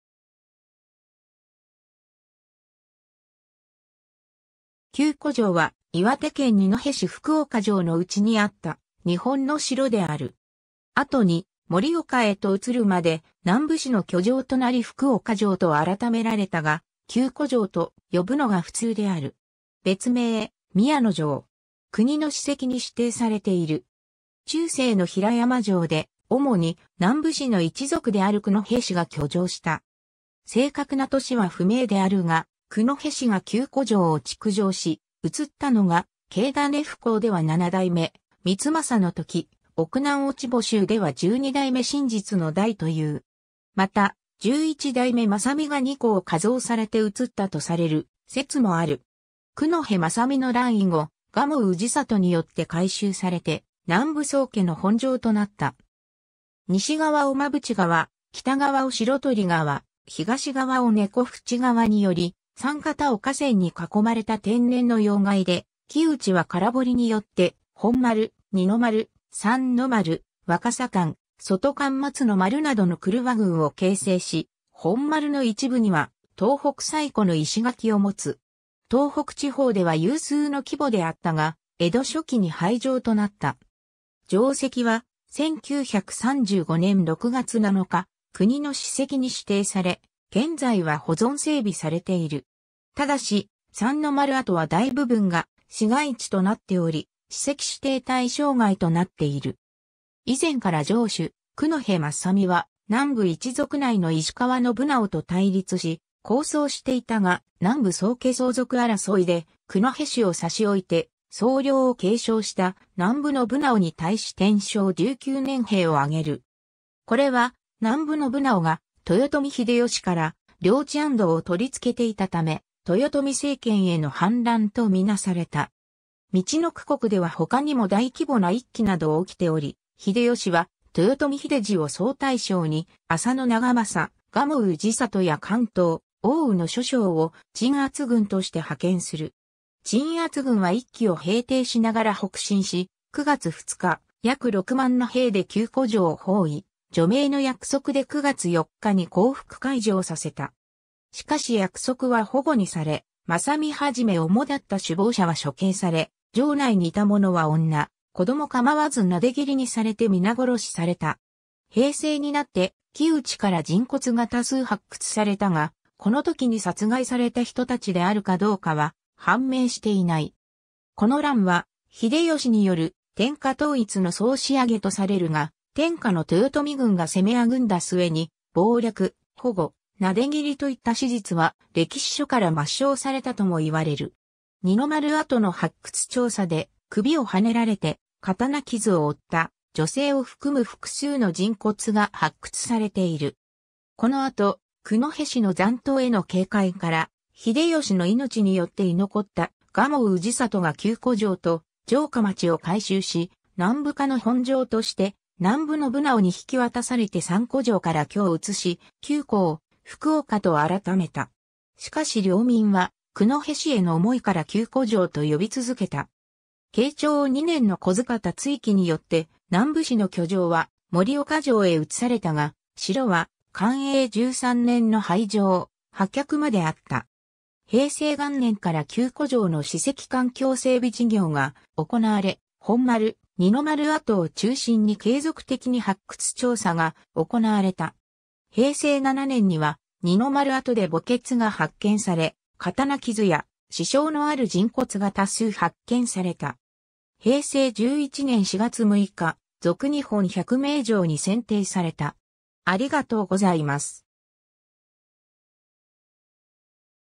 九戸城
戦国時代_(日本)
九戸城（くのへじょう）は、岩手県二戸市福岡城ノ内にあった日本の城である。
後に盛岡へと移るまで南部氏の居城となり福岡城と改められたが、九戸城と呼ぶのが普通である。
別名「宮野城」。
 中世の平山城で、主に南部氏の一族である九戸氏が居城した。
正確な年は不明であるが、九戸氏が九戸城を築城し移ったのが、『系胤譜考』では7代目光政のとき、『奥南落穂集』では12代目信実の代という。
また、11代目（『奥南落穂集』の代数では14代目）政実が二戸を加増されて移ったとされる説もある。
 「九戸政実の乱」以後、蒲生氏郷によって改修されて南部宗家の本城となった。
 西側を馬淵川、北側を白鳥川、東側を猫渕川により、三方を河川に囲まれた天然の要害で、城内は空堀によって、本丸、二の丸、三の丸、若狭館（わかさだて）、外館（とだて、石沢館とも）松の丸などの曲輪群を形成し、本丸の一部には東北最古の石垣をもつ。
東北地方では有数の規模であったが江戸初期に廃城となった。
 城跡は、1935年（昭和10年）6月7日、国の史跡に指定され、現在は保存整備されている。
ただし、三の丸跡は大部分が市街地となっており、史跡指定対象外となっている。
 以前から城主・九戸政実は、南部一族内の石川（南部）信直と対立し抗争していたが、南部宗家相続争いで九戸氏を差し置いて惣領を継承した南部信直に対し天正19年（1591年）兵を挙げる。
これは、南部信直が豊臣秀吉から領地安堵をとりつけていたため豊臣政権への反乱とみなされた。
陸奥国では他にも大規模な一揆など起きており、秀吉は豊臣秀次を総大将に浅野長政、蒲生氏郷や関東、奥羽の諸将を鎮圧軍として派遣する。
鎮圧軍は一揆を平定しながら北進し、9月2日約6万の兵で九戸城を包囲、助命の約束で9月4日に降伏開城させた。
しかし約束は反故にされ、政実はじめ主だった首謀者は処刑され、城内にいた者は女、子供構わず撫で斬りにされて皆殺しされた。
平成になって城内から人骨が多数発掘されたが、この時に殺害された人たちであるかどうかは判明していない。
 この乱は、秀吉による天下統一の総仕上げとされるが、天下の豊臣軍が攻めあぐんだ末に謀略、反故、撫で斬りといった史実は歴史書から抹消されたともいわれる。
二ノ丸跡の発掘調査で、首を刎ねられて刀傷を負った、女性を含む複数の人骨が発掘されている。
この後、九戸氏の残党への警戒から、秀吉の命によって居残った蒲生氏郷が九戸城と城下町を改修し、南部家の本城として南部信直に引き渡されて三戸城から居を移し、九戸を福岡と改めた。
しかし領民は九戸氏への思いから九戸城と呼び続けた。
 慶長2年（1597年）の不来方（盛岡）築城によって南部氏の居城は盛岡城へ移されたが、城は寛永13年（1636年）の廃城、破却まであった。
 平成元年（1989年）から九戸城の史跡環境整備事業が行われ、本丸・二ノ丸跡を中心に継続的に発掘調査が行われた。
平成7年（1995年）には二ノ丸跡で墓穴が発見され、刀傷や刺傷のある人骨が多数発見された。
平成11年（1999年には二ノ丸跡で工房の遺構が発見され、鎧の札などが発見された。これらの調査で多くの出土品が発見されて、九戸氏が近隣だけではなく日本の中心である京都や大阪等の中央とも交流があり、交易が行われていたことが裏付けられた。 平成29年（2017年）4月6日、続日本100名城（104番）に選定された。